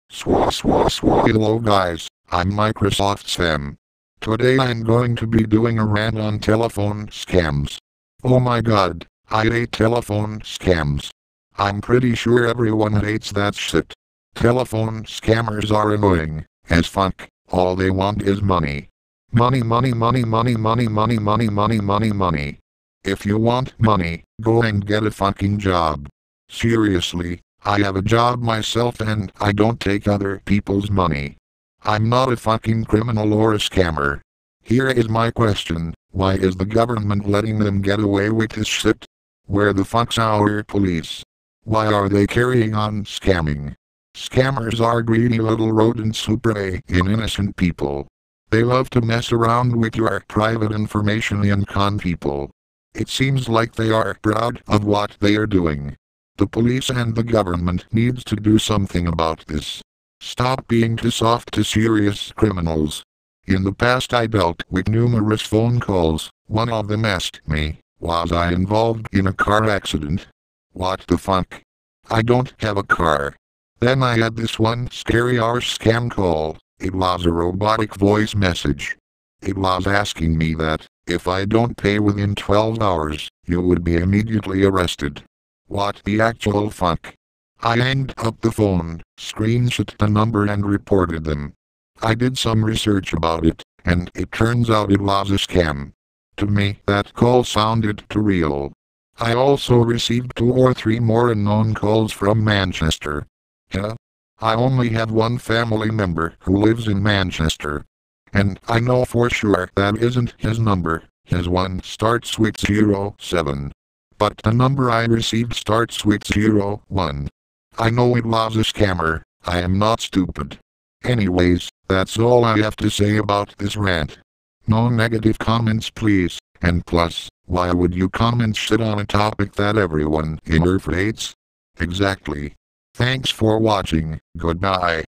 Rant on telephone scams
The government needs to be stricter with serious crimes loke this. Telephone scammers need to be punished more than pampered!